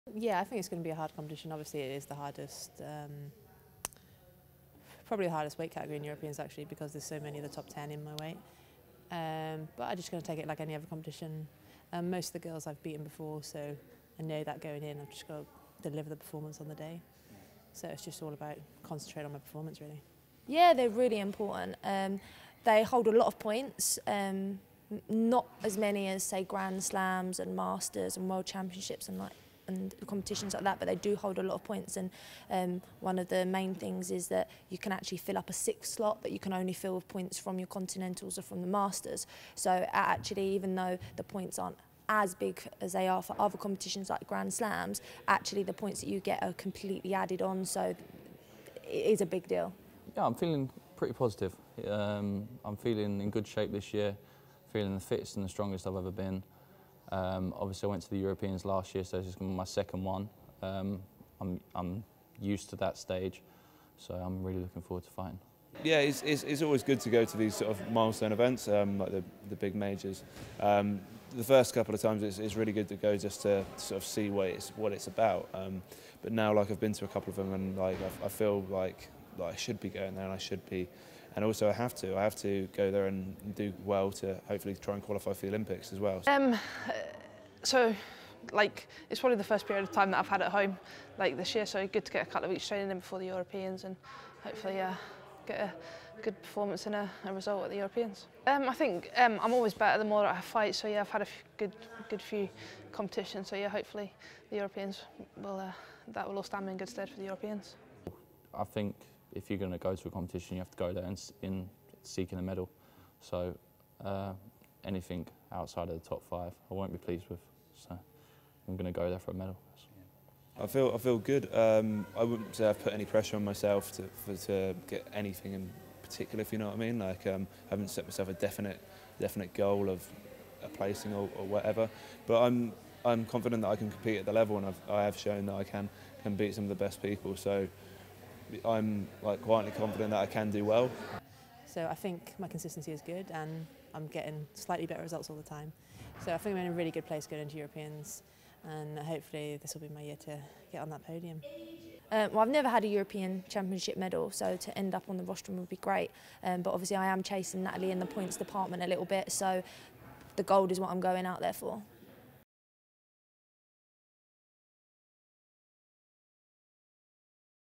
2016 European Championships: Gibbons, Powell, Adlington, Chamberlain & Fletcher
Gemma Gibbons, Natalie Powell, Sarah Adlington, Frazer Chamberlain and Ben Fletcher give us their thoughts ahead of the 2016 European Championships in Kazan, Russia 21-24 April